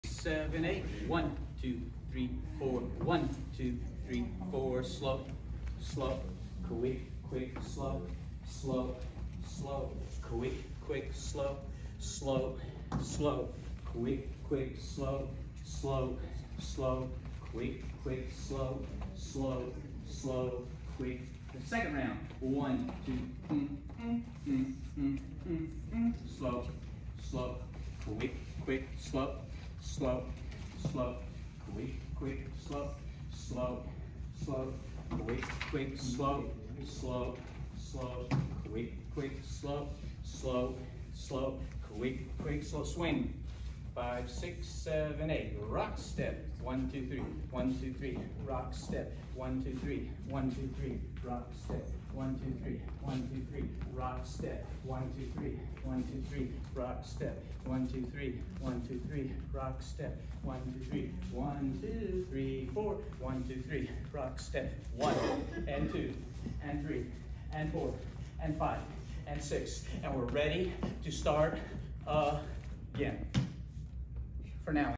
September 2024 Tango and Triple Swing
End of the Month summary of steps in Intermediate Tango and Triple Swing